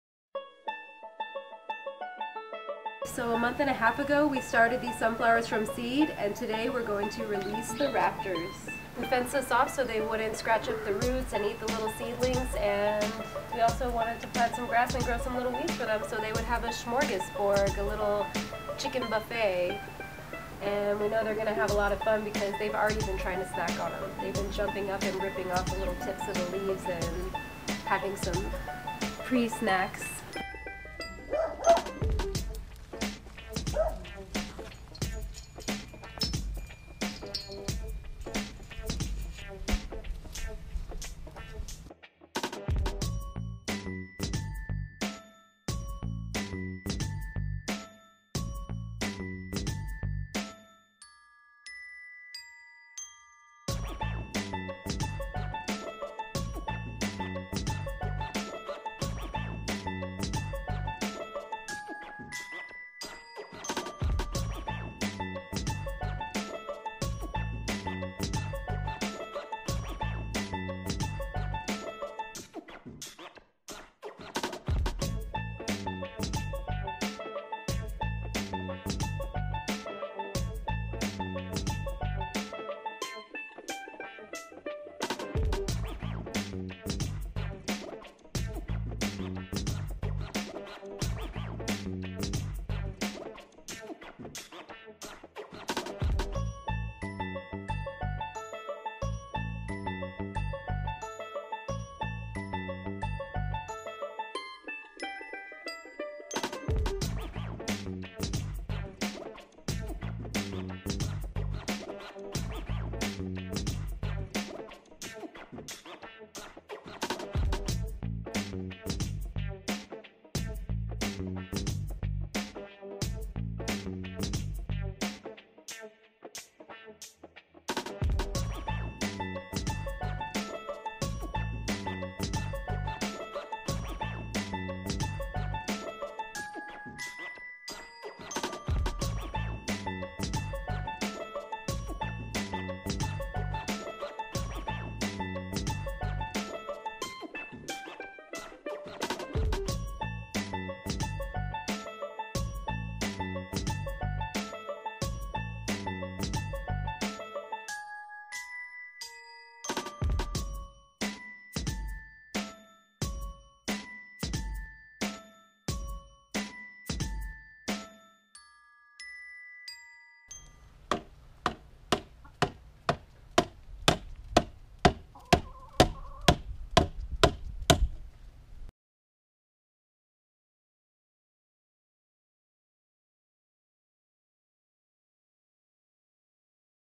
How To Keep Chickens Out of Your Garden ***WAIT TILL THE END***
How To Keep Chickens Out of Your Garden. In the beginning we show you how to temporarily chicken proof the garden with a simple chicken wire fence, at the end we show you how to effectively protect your garden and keep chickens from destroying your plants permanently. (Joke) We planted sunflowers and grass for our chickens and made a chicken buffet for them to enjoy. We custom made the sign shown at the end =]

OUR AMAZON STOREFRONT
(All our FAVORITE Amazon products in one place!)

👕Shop our BockBock T-shirts and more!